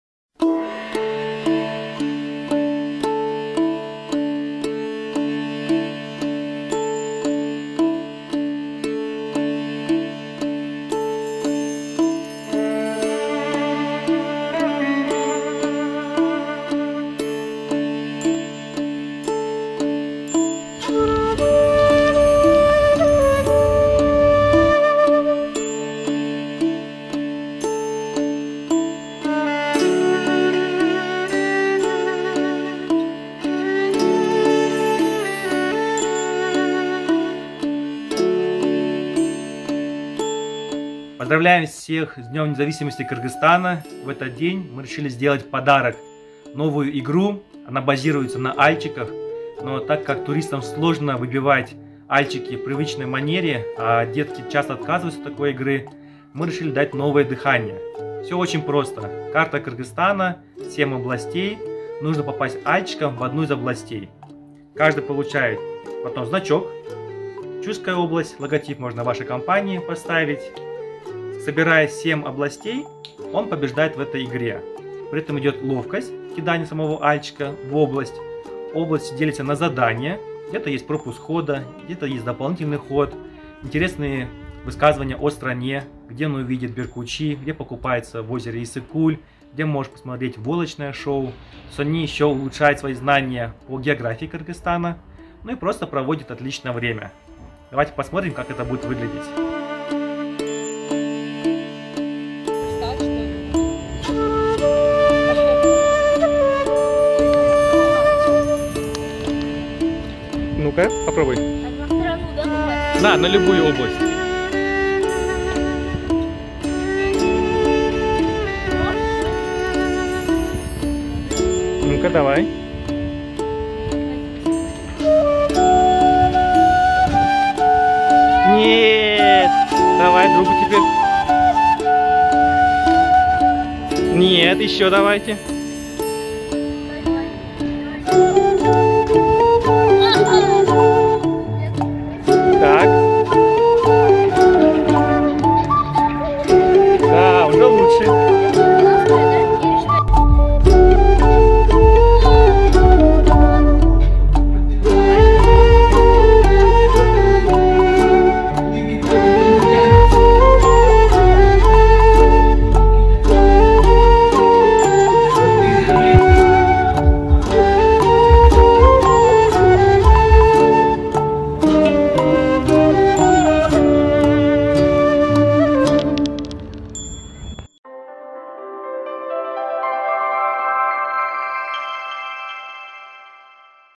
31 августа 2022 г.
На праздник принято дарить подарки : )

Мы решили подарить новую игру!

Она базируется на с детства любимых альчиках, но им придали новое дыхание! Туристам и маленьким мальчикам сложно играть в альчики в обычной манере. Поэтому придумали более простой вариант по технике, но при этом еще и позновательный для изучения географии Кыргызстана!

Участники по очереди кидают альчики на карту страны. Их задача попасть на все 7 областей, за каждую из них получют фишку. Тот, кто быстрее соберет все 7 фишек – победил!

Плюс есть различные задания, в зависимости, на какую цифру попадут. Пропуск хода, доп. ход, станцевать, спеть, и т.д. Так же узнают, где проживают беркутчи, где изготавливают войлочные ковры и т.д. Будут играть и знания получать!

Ну а для профи – супер игра! На лого ставятся альчики соперников и если победитель сбивает один из них с трех попыток, то проигравшие выполняют любое его желание!

Цена игры зависит от размера карты. Большой вариант, как на видео, стоит 3 333 сом. Включает карту размером 1.4м на 2.7м, 28 фишек областей (7х4), 4 альчика. По желанию на карту и фишки можно нанести логотипы вашей компании или имена туристов.

Вместе сделаем туризм лучше!

“TRAVEL EXPERTS” – the best tours!